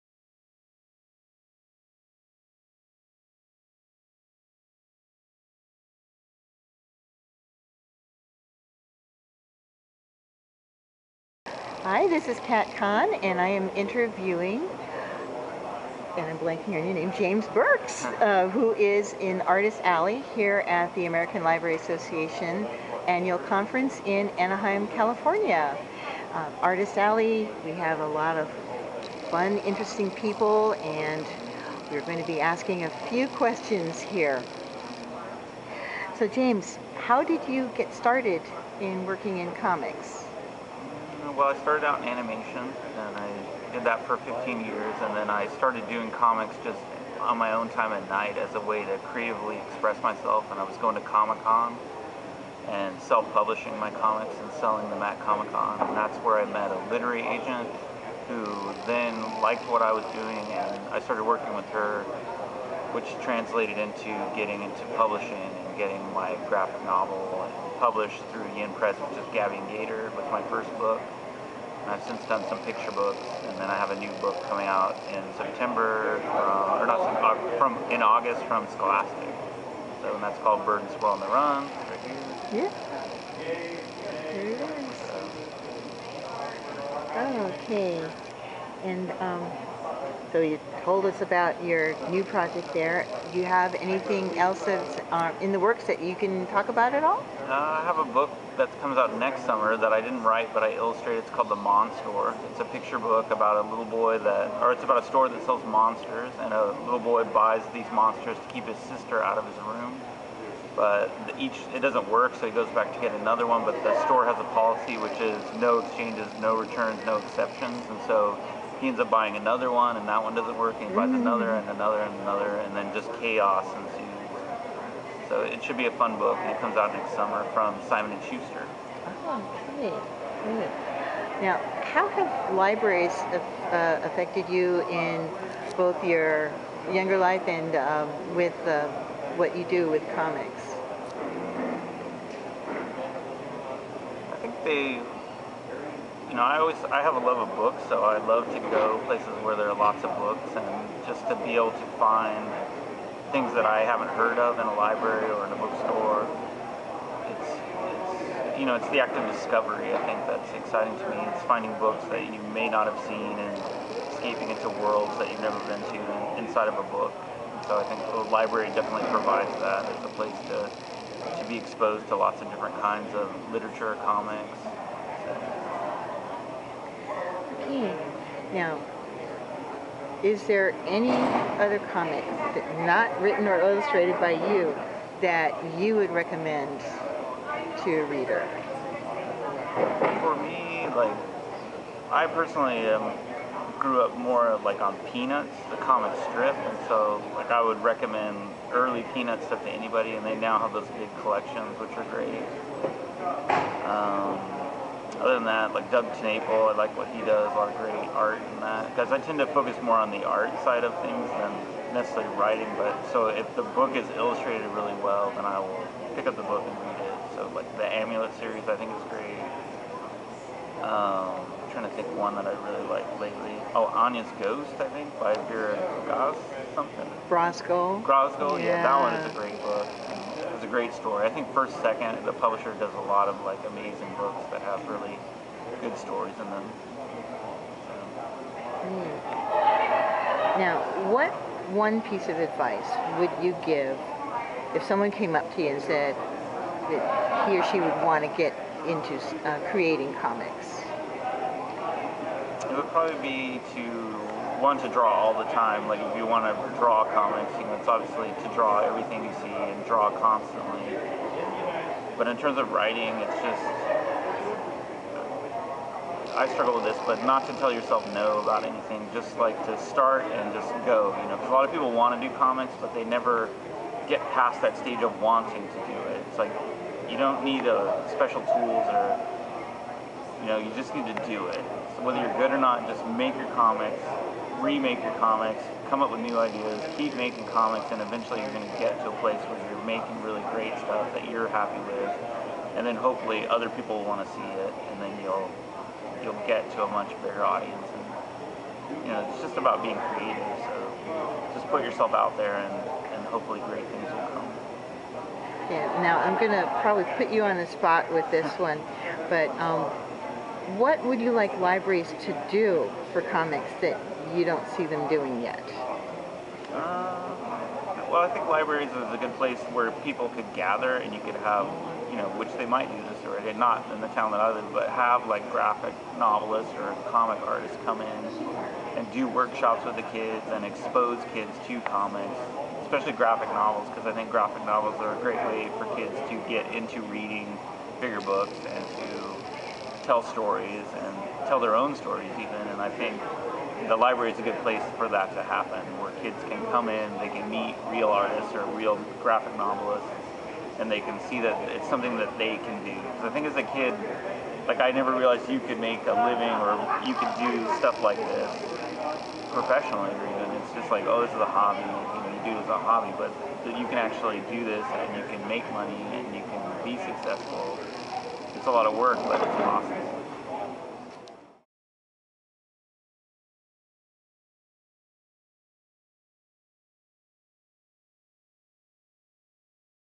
Artist Alley: James Burks
Kat Kan interviews James Burks at the 2012 ALA Annual Conference about his start in animation and comics, his current projects, his love of Charles Schulz's Peanuts, and his advice for aspiring artists.